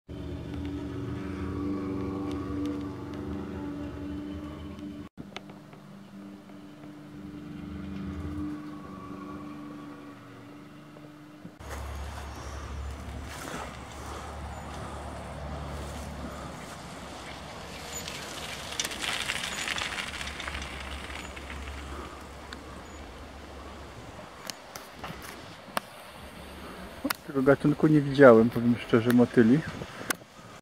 Koszenie trawy pod blokiem oraz taniec motyli na działce.
Przez lata do koszenia używano kosy spalinowej,teraz powrócono do dawnych czasów używając kosiarki. Na działce pojawiły się rzadko spotykane u mnie gatunki motyli w tańcu godowym. Zazwyczaj dominuje rusałka pawik i bielinek.